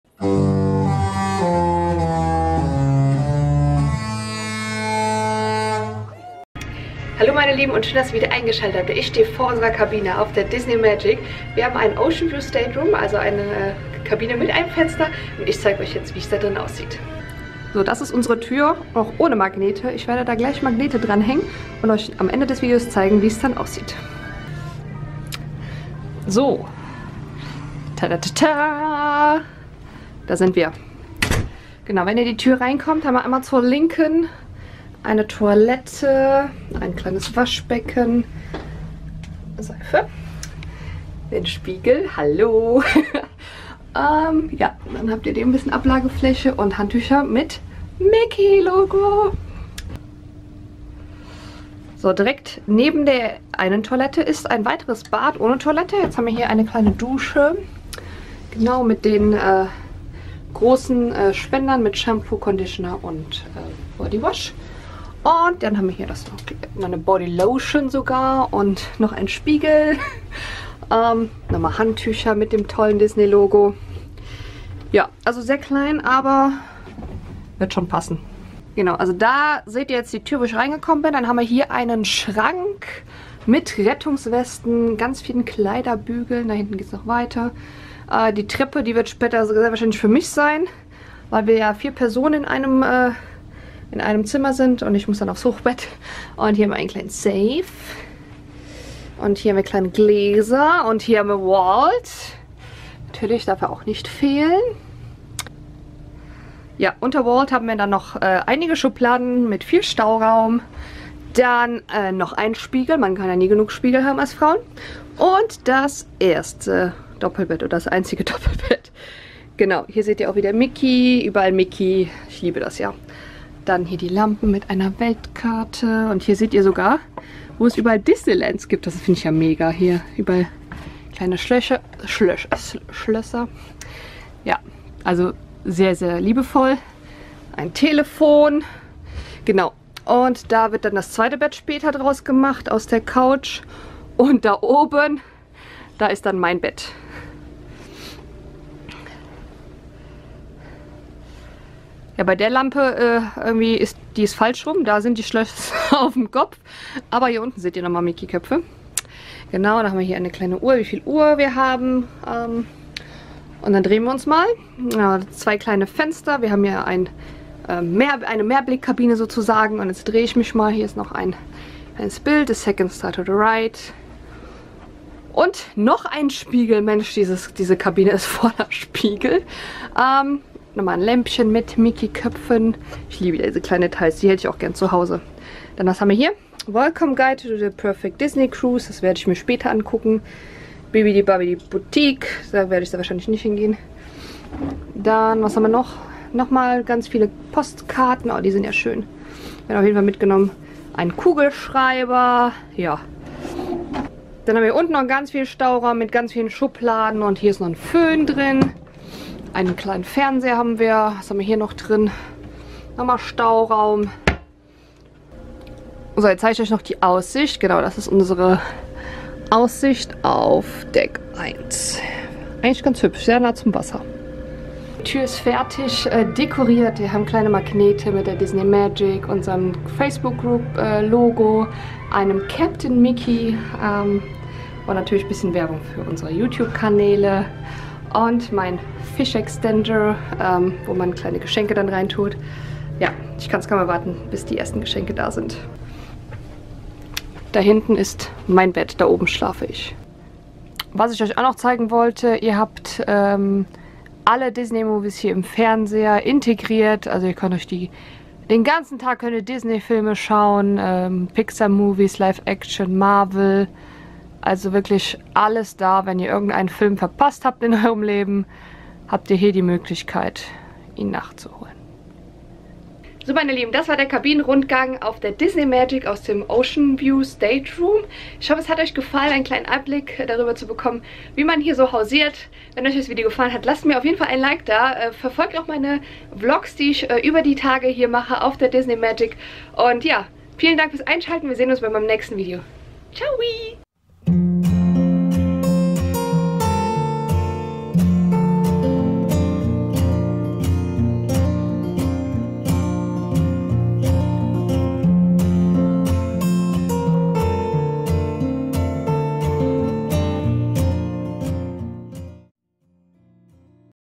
Deluxe Oceanview Stateroom - Kabinentour auf der Disney Magic
Hallo an alle Disneyfans,

Willkommen zu meiner Roomtour auf der Disney Magic. In diesem Video zeige ich euch wie eine Meerblickkabine auf der Disney Magic aussieht! Ich hoffe ihr habt Spaß beim zuschauen!

Auch zukünftig möchte ich euch in meine Welt von Disney mitnehmen und euch zeigen wie es sich in mein Leben integriert!
(Dieses Video enthält unbezahlte Werbung durch Markenerkennung oder Personenmarkierungen)

disney_withsimone